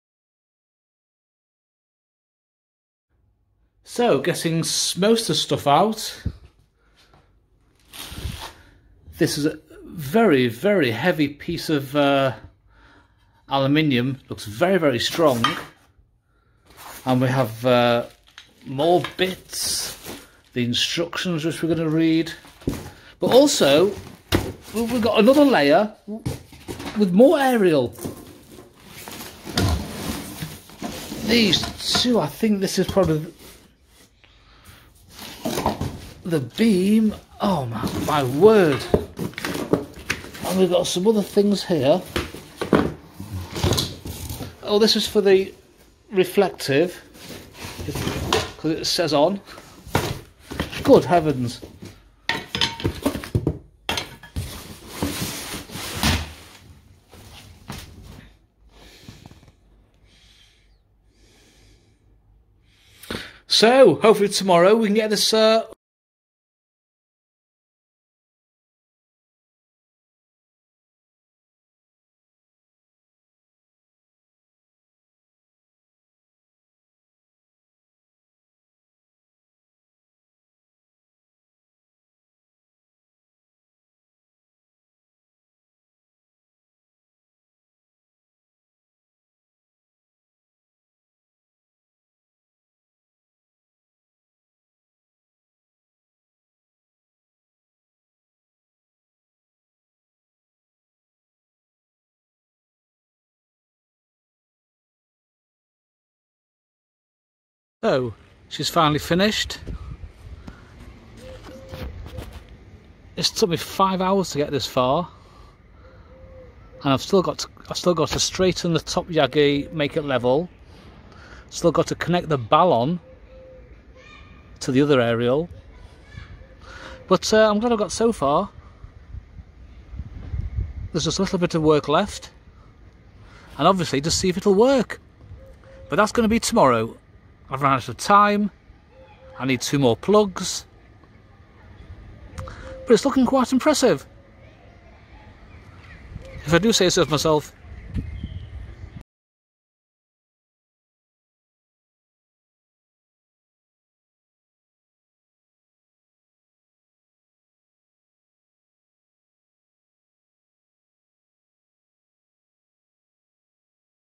STACKING A 11 METER 4 ELEMENT YAGI OVER A 3 ELEMENT 3 BAND YAGI 🤔🤔🤔PART 1
Hi,since I passed the Amateur Radio Exam 🙃, I have a 3 element, 20,15,10 meter yagi, but the swr is definitely not for the cb frequencies, so I am trying stacking the 11 metre 4 element on top of the other yagi 🙃, some operators say it can and other operators say it most definitely cannot 🤔🤔,tomorrow when I finish, we will find out,,,,but I am quietly confident 😀,,,,,part 2 soon,,,,thanks from Rotterdam 73. PART 2 SOON.